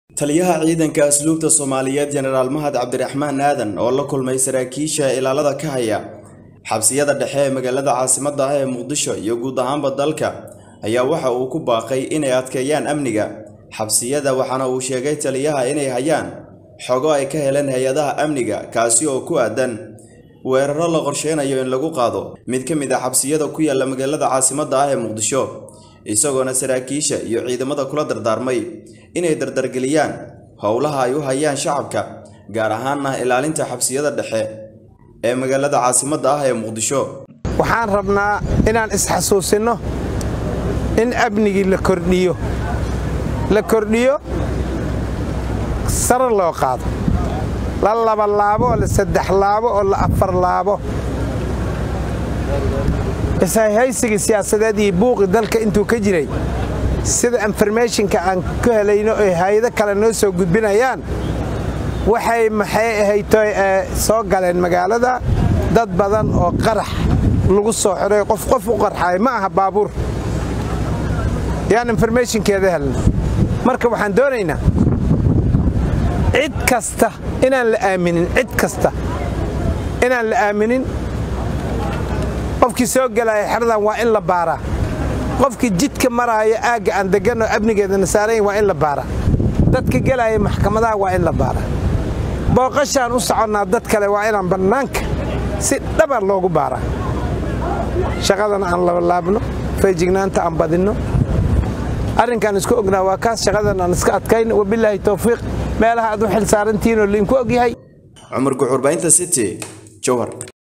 Taliyaha Ciidanka Asluubta Soomaaliyeed ayaa sheegay inay Jiraan Xogo sheegaya
Taliyaha Ciidanka Asluubta Soomaaliyeed ayaa sheegay inay Jiraan Xogo sheegaya in weerar lala damacsan yahay Mid kamid ah Xabsiyada ay ilaaliyaan Ciidanka Asluubta BY Wariye Cumar Guux